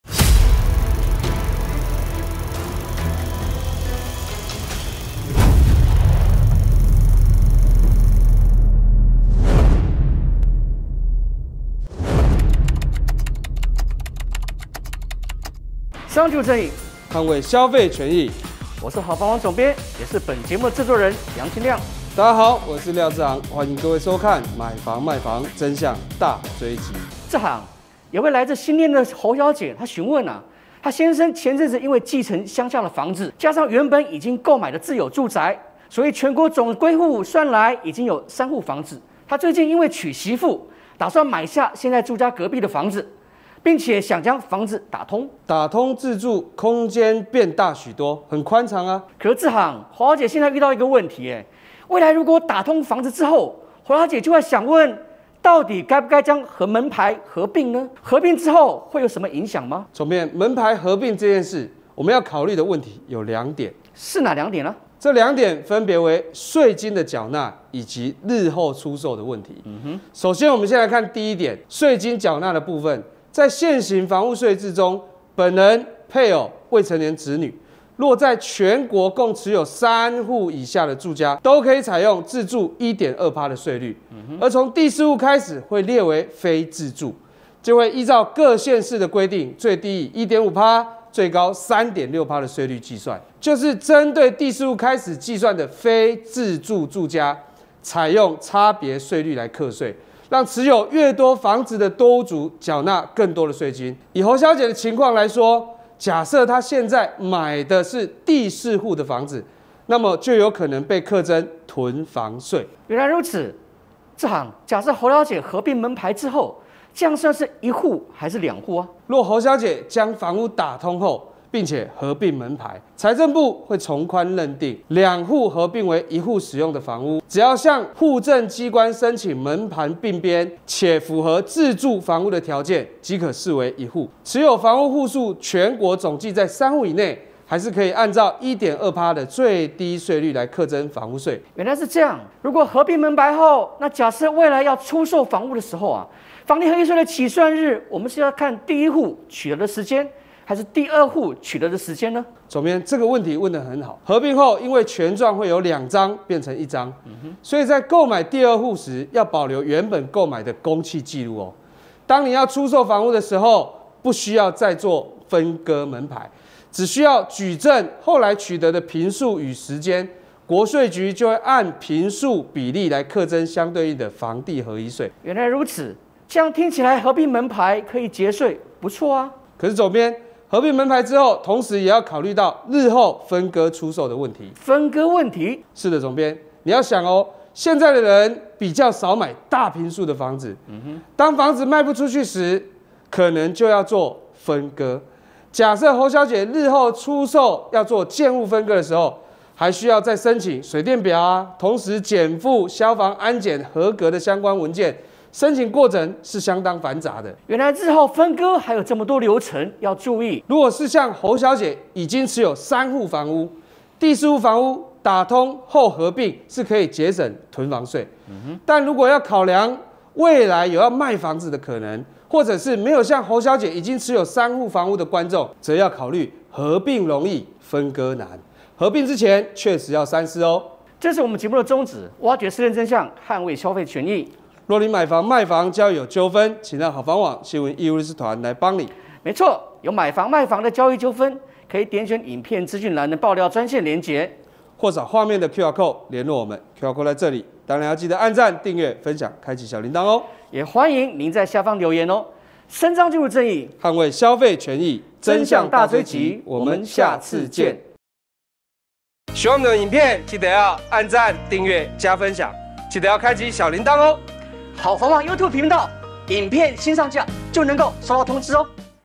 【好房網TV】《買房賣房真相大追擊》房子打通要算一戶還是兩戶？想清楚！合併容易分割難｜楊欽亮、廖志航主持
新店侯小姐最近買下隔壁的房子，
要不要把門牌合而為一很讓她傷腦筋。
合併門牌有哪些優點？不合併呢？
在選擇之前，建議你考量兩件事。
一是稅金，
此時新買的房子是不是第四戶會是考量重點，為什麼？
二是將來要不要賣，
合併門牌，不合併門牌，轉售時有差嗎？
另外，出售房屋時的房地合一稅，要從買進哪一戶時起算呢？
這些問題，讓專家回答你！

延伸相關影片：
歡迎網友在影片留言區 分享您的看法
為居住正義勇敢發聲！
有中古屋買賣糾紛，請跟好房網爆料